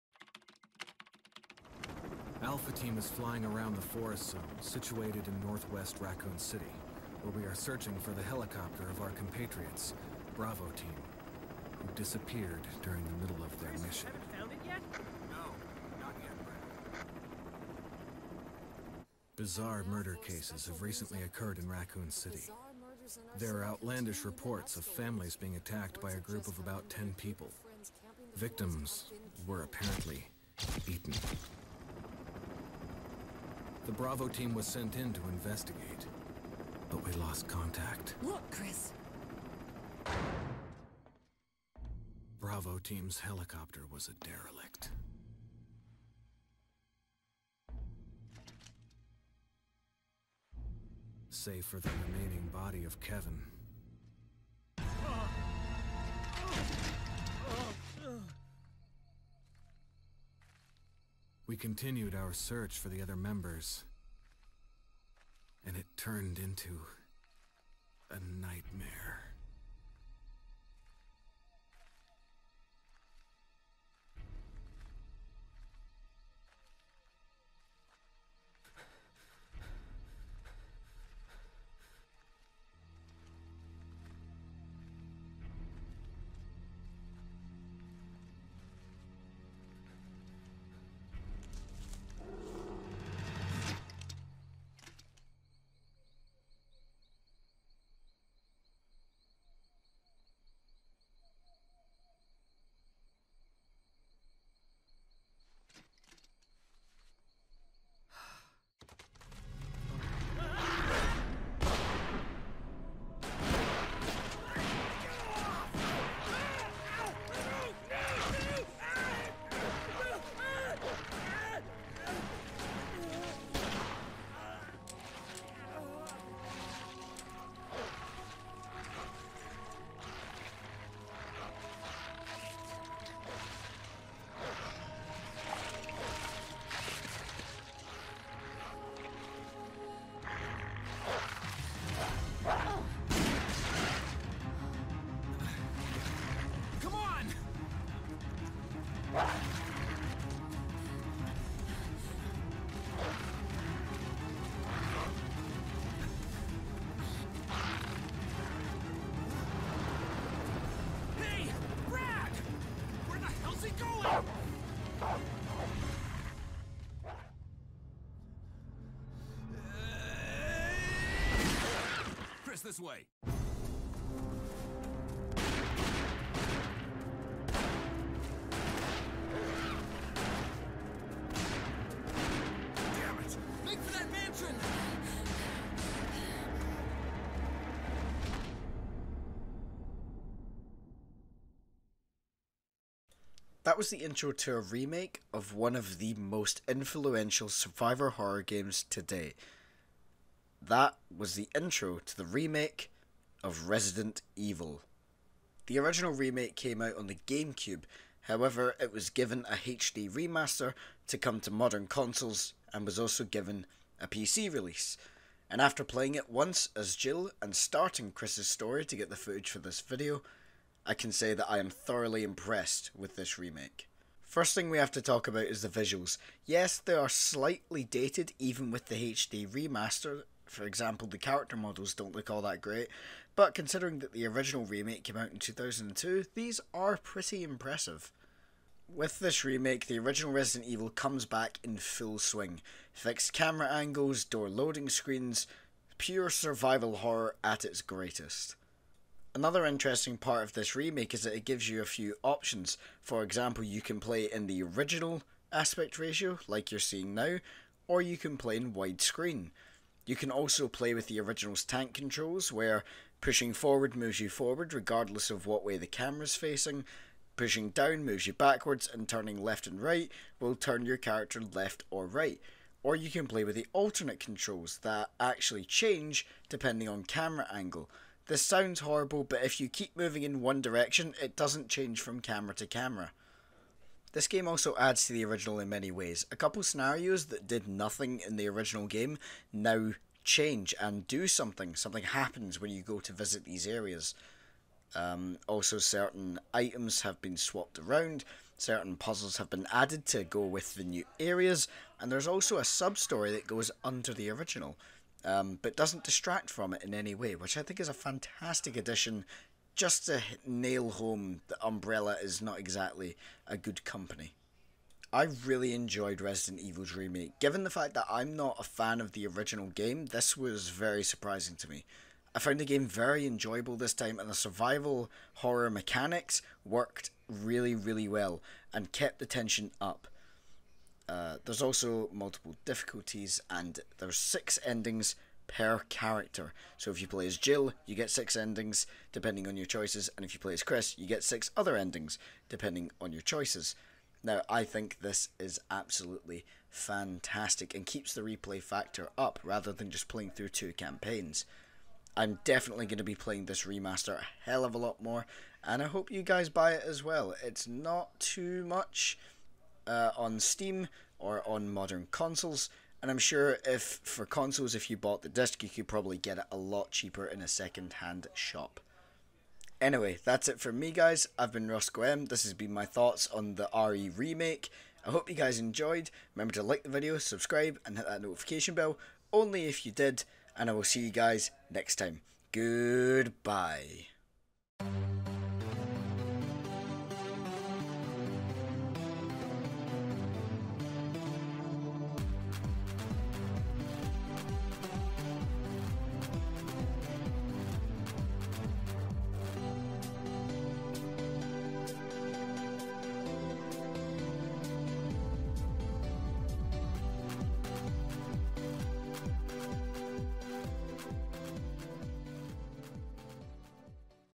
Resident Evil Remake Review
I hope you guys enjoyed this video, in this one I give my thoughts on the Remake of Resident Evil. Enjoy!